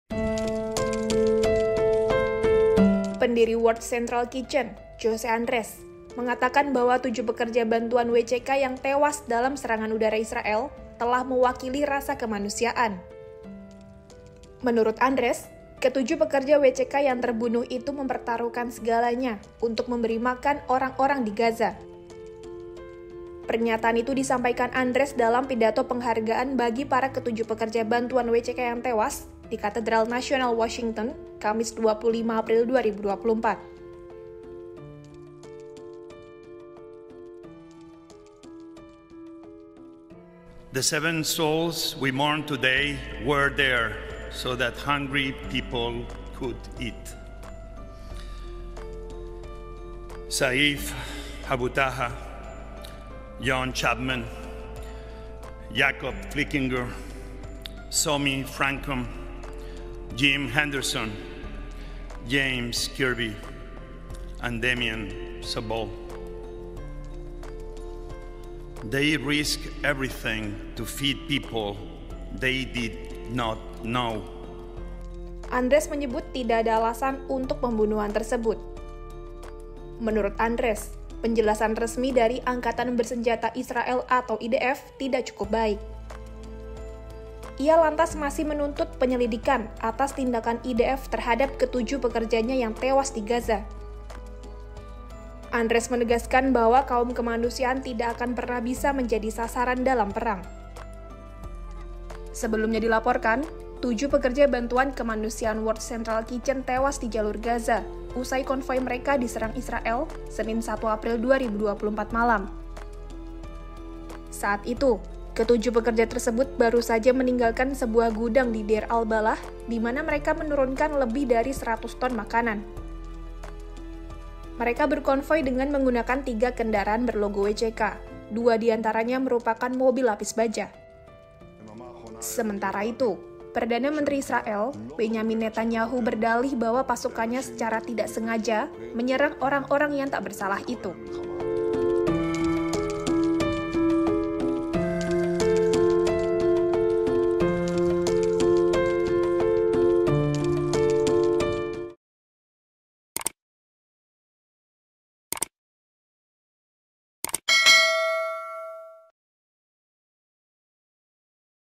7 Relawan Bantuan Kemanusiaan yang Dibunuh Israel Dapat Penghormatan
Pendiri World Central Kitchen (WCK), Jose Andres, memberikan penghormatan bagi tujuh relawan bantuan kemanusiaan yang terbunuh dalam serangan udara Israel di Gaza.

Andres menyebut, ketujuh pekerja bantuan itu mewakili rasa kemanusiaan untuk Gaza. Ia menambahkan, tujuh relawan WCK telah mempertaruhkan segalanya untuk memberi makanan warga Gaza.

Andres mengatakan tidak ada alasan atas pembunuhan tersebut dan dia menyerukan penyelidikan atas kematian itu.

Sebelumnya, tujuh pekerja bantuan kemanusiaan dari World Central Kitchen  tewas di Jalur Gaza setelah konvoi mereka diserang pasukan Israel pada Senin (1/4/2024) malam.

Penulis Naskah: Vina Muthi Ambarwati
Narator: Vina Muthi Ambarwati
Video Editor: Maria Dewi
Produser: Ervan Yudhi Tri Atmoko